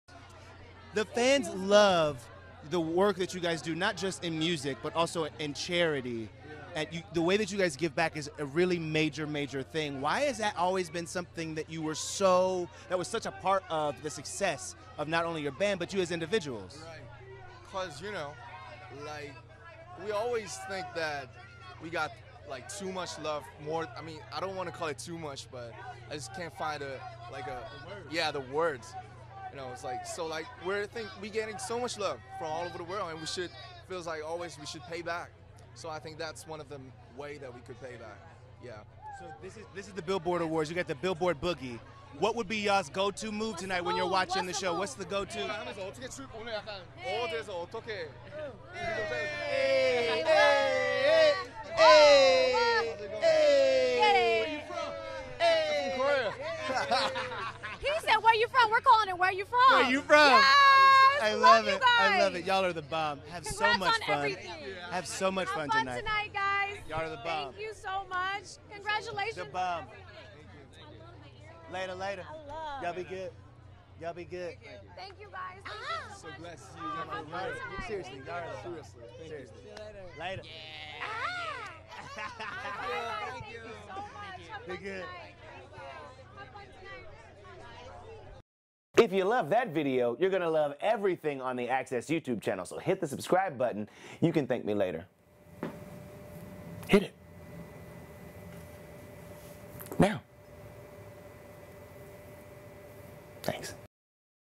BTS Show Off Impressive Dance Moves & Talk Giving Back To Charity | Access
BTS (방탄소년단) chats with Access at the 2019 Billboard Music Awards and shares why giving back to charity is so important to them. Plus, they bust some impressive moves on the red carpet.

BTS Show Off Impressive Dance Moves & Talk Giving Back To Charity